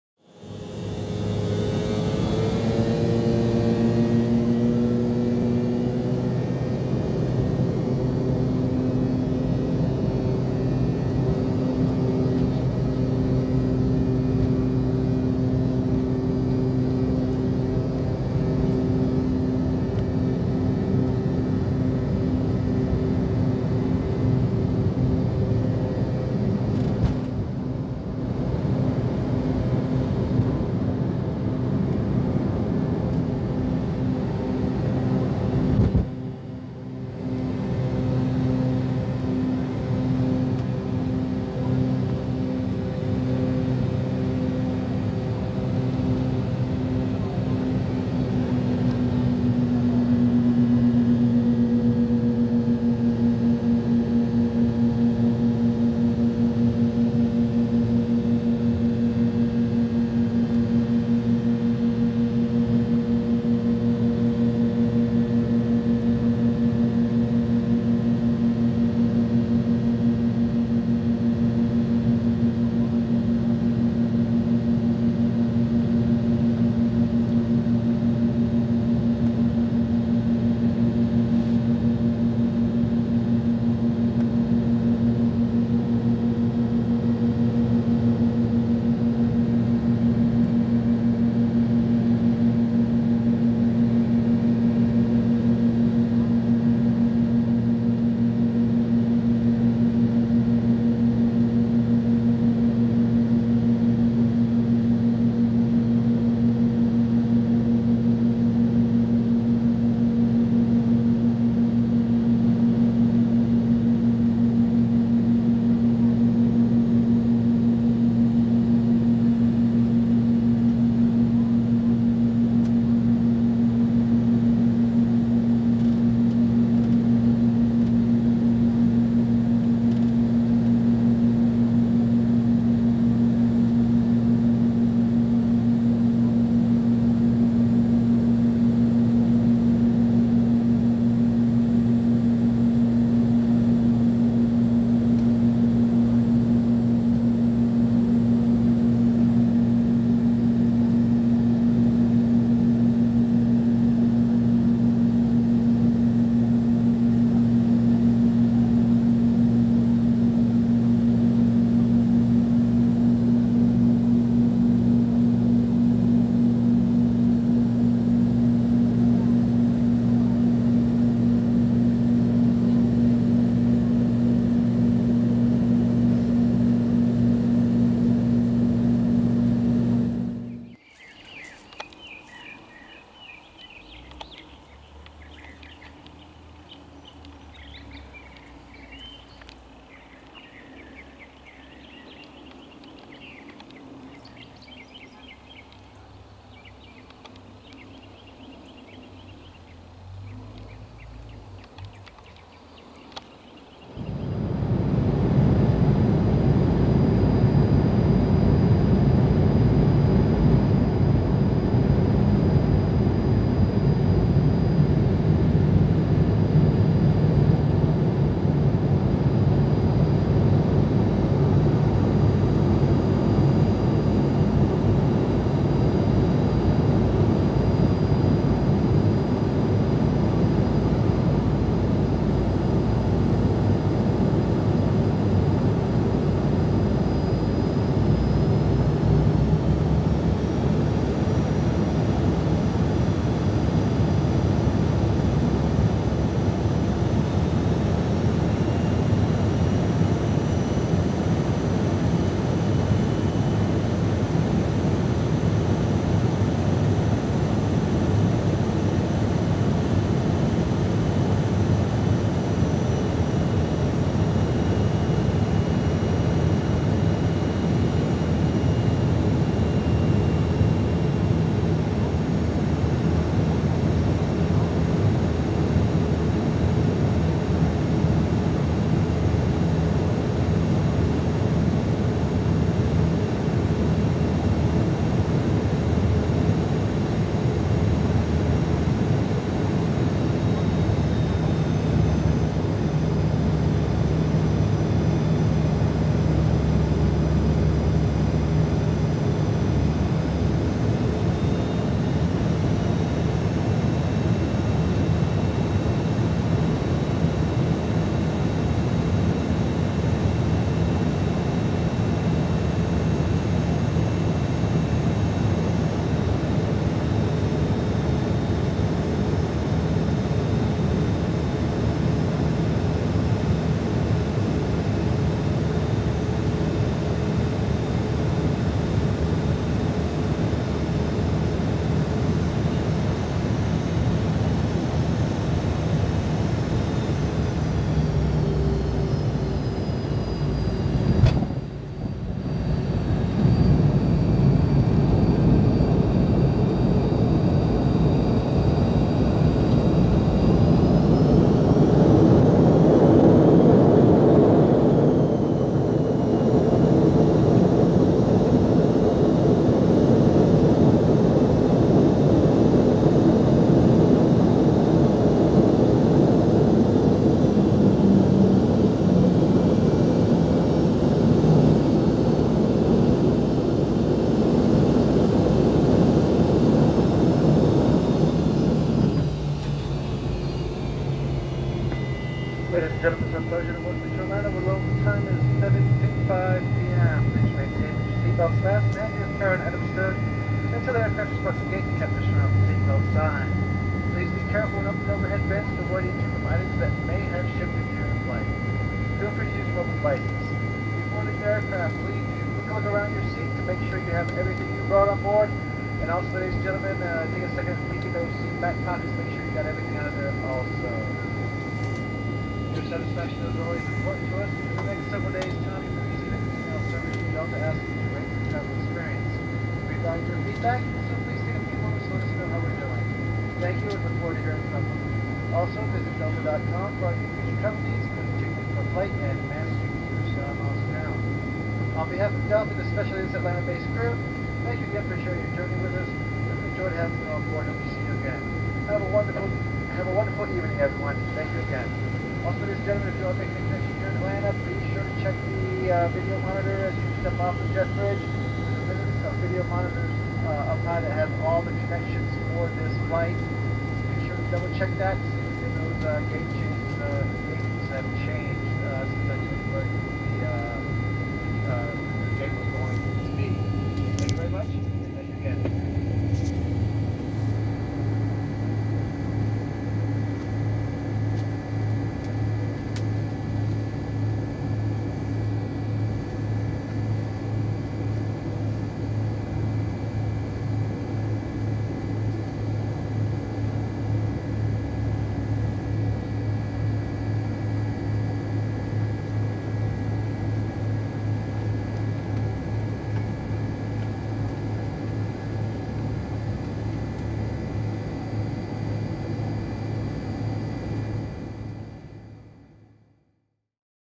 KORF to KATL | MD88 | Delta Airlines Flight 860
Aircraft type - McDonnell Douglas MD88
Departure airport - KORF
Crusing speed - 428kt (493mph) ground speed
Crusing Altitude - FL340 (34,000ft)
STAR (Standard Terminal Arrival Route) - HOTHH.JJEDI2
Landing runway - 28
Arrival airport - KATL
Shoutout to my mom for the recordings.

New video with Turkish Airlines coming up soon. I'll be posting the flights separate.